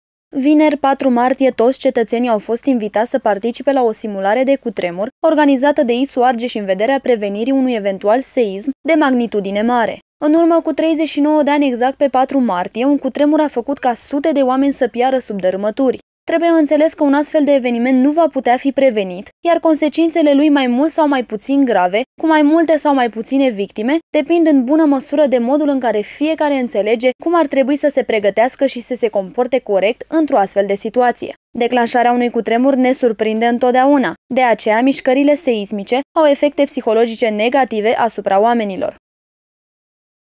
Stire 04 03 2016 Cutremur ISU Arges
În urmă cu 39 de ani, pe 4 Martie 1977, un devastator cutremur zguduia România. Atunci au pierit sub dărâmături sute de oameni, printre ei aflându-se şi personalităţi importante ale vieţii artistice şi culturale româneşti. Pentru a marca acest dezastru, astăzi, ISU Argeş a organizat o simulare de cutremur.